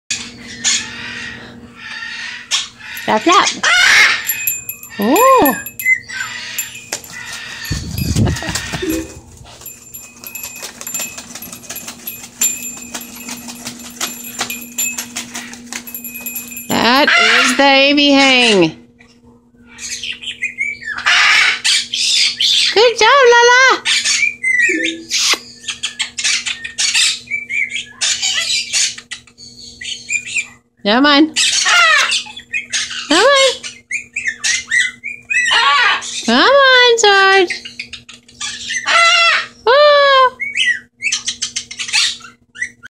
AviHang for little birds but Macaws like it too!
Swing by Nyla Copp, Macaw baby hand raised by Inca's Secret Parrot Toys and Boarding in Kent, WA.